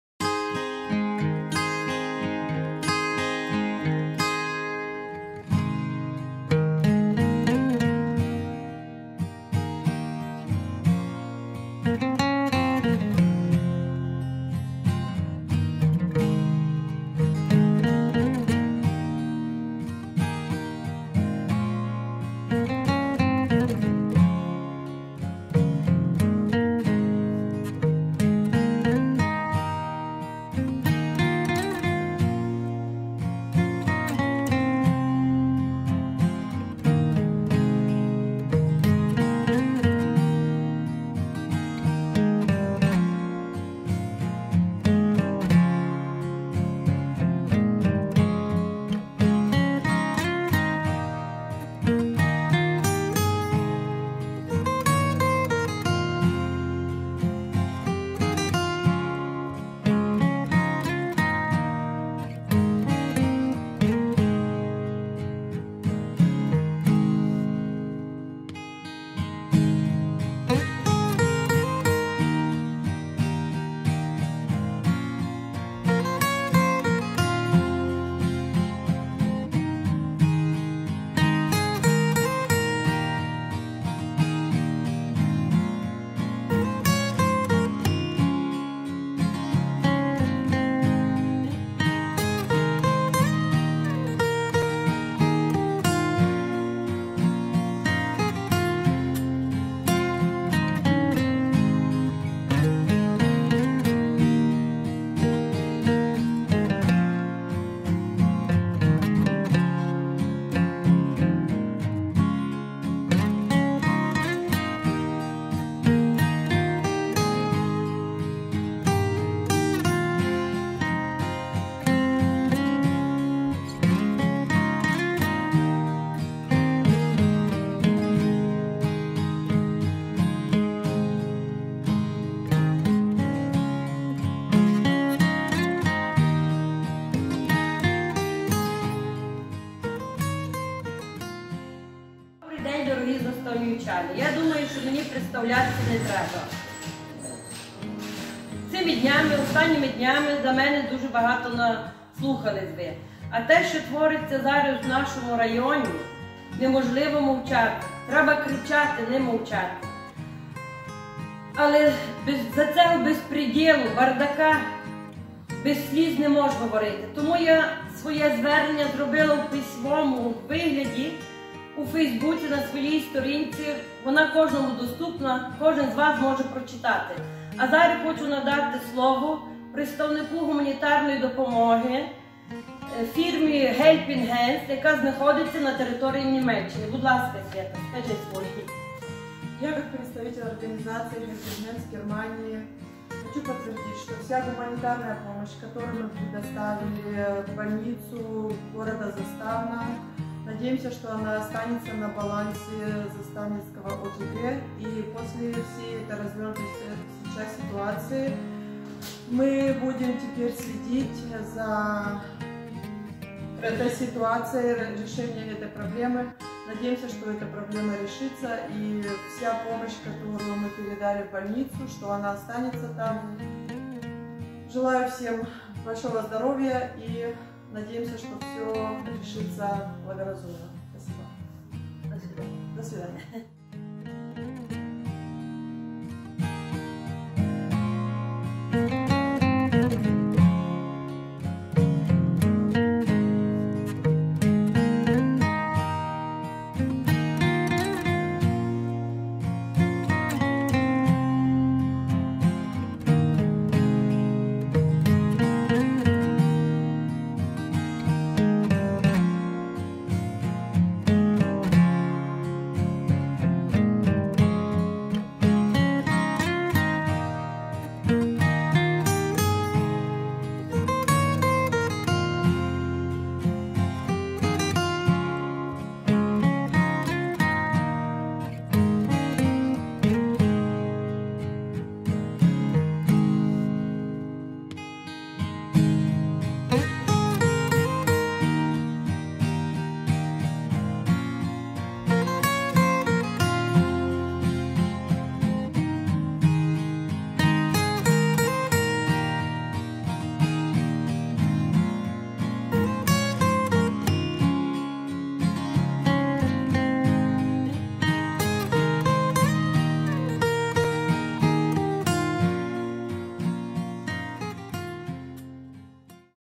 Точка зору: наші благодійники про розподіл майна міста Заставна
Людмила Ефкен, почесна жителька міста Заставна, записала письмове та відеозвернення, обурена рішеннями сесії районної ради від 6-7 листопада.
Щиро дякуємо пані Людмилі за підтримку!